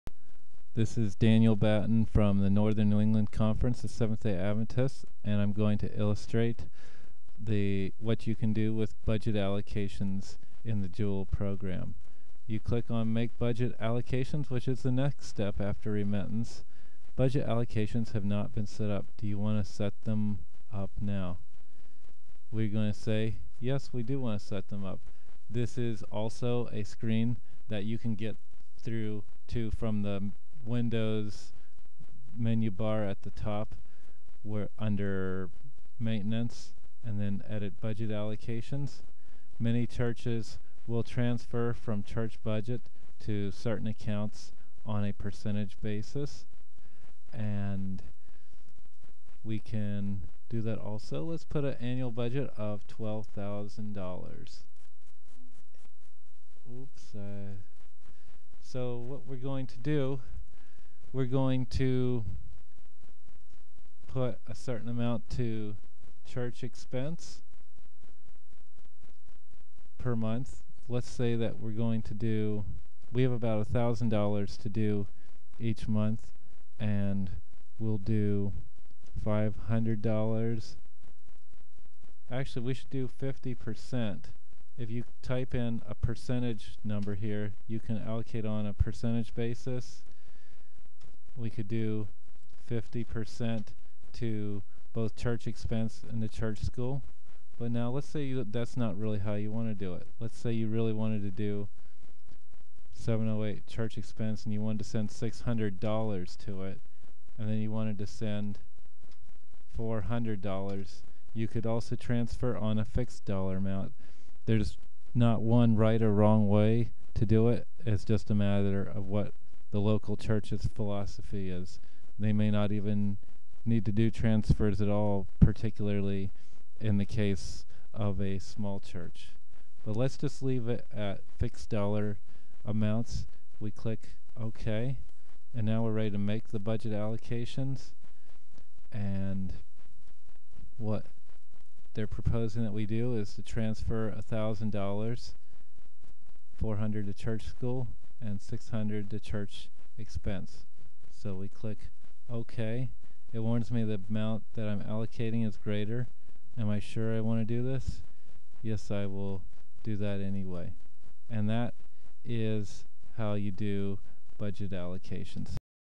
JewelBudgetAllocations
How to do budget allocations in the Jewel SDA Accounting Program